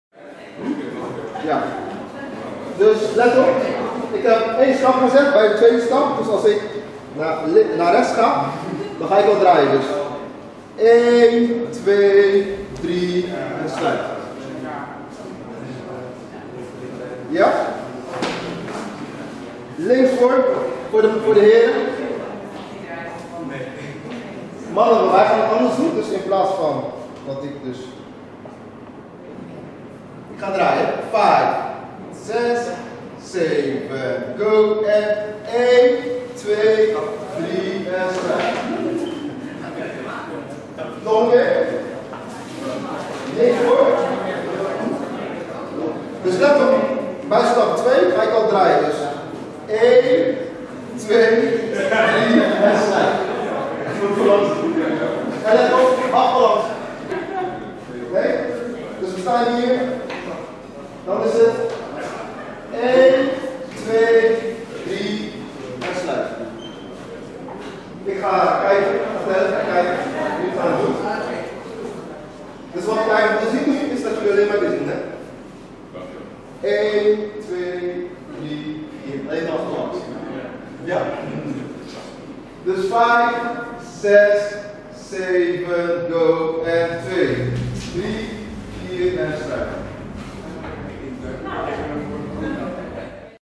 Workshop The Turning Movement Semba Dance Daniel & Rianda Holland Kizomba Congress
Workshop The Turning Movement Semba Dance
 Daniel & Rianda Holland Kizomba Congress